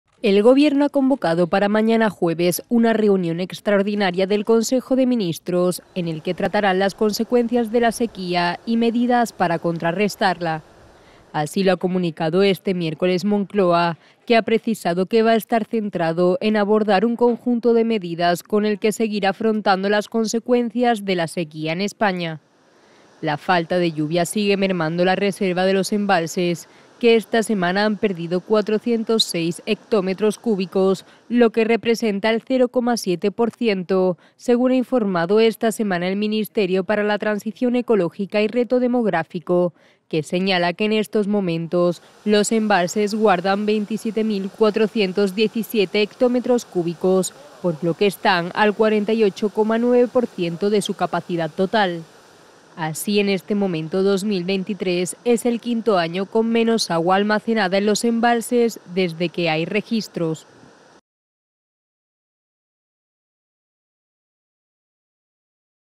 Sánchez riega el campo de millones ante el temor a la venganza de la España rural el 28-M
El presidente convoca un Consejo de Ministros extraordinario para este jueves -del que Podemos no sabía nada- con la intención de aprobar una batería de medidas para paliar la sequía

Vídeo: EP